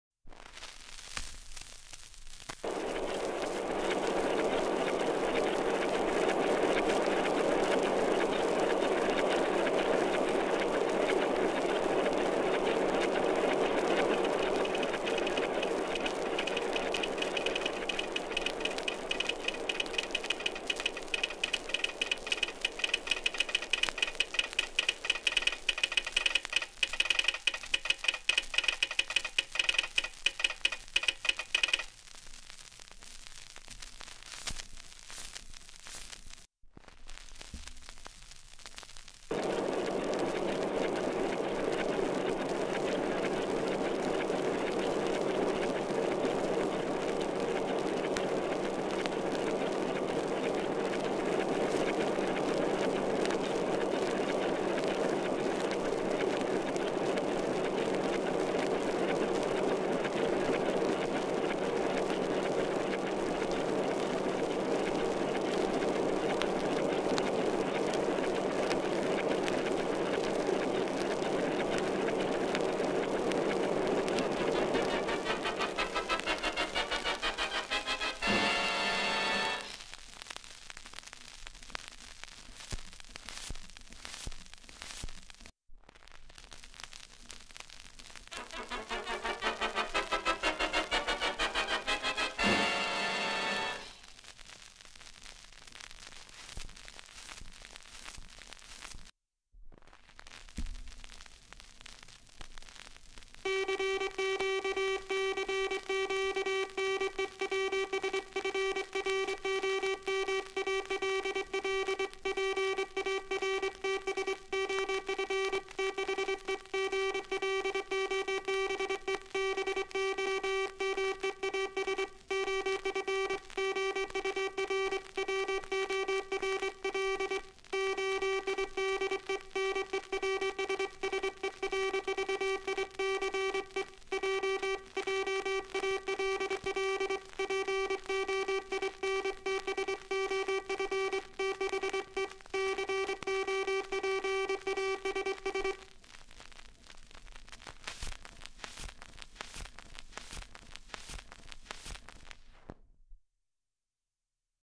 Archival News Broadcasting Effects From Standard Sound Effect Part 2 (10K Subscribers Special)
Digitized at 78 revolutions per minute. Four stylii were used to transfer this record. They are 3.8mil truncated conical, 2.3mil truncated conical, 2.8mil truncated conical, 3.3mil truncated conical. These were recorded flat and then also equalized with Turnover: 250.0.